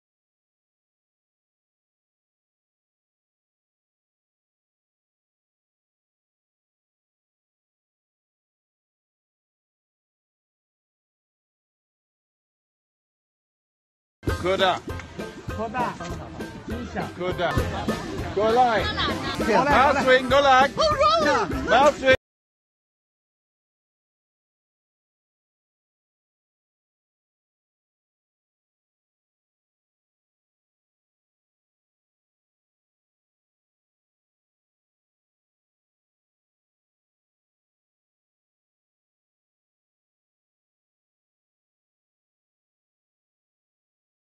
Thranduil becomes a panda babysitter? Movie star Lee Pace visits giant pandas in Chengdu
The internationally known actor Lee Pace appeared in southwest China’s Chengdu together with pandas on February 21, 2017. Invited by Chengdu Research Base of Giant Panda Breeding (CRBGPB) and Conservation International (CI), Pace showed up at the breeding center for the Chengdu Giant Panda Nursing Experience. The actor even picked up some Sichuan dialect and used it on the giant pandas when feeding them just like how professional panda babysitters would do. The video went viral online.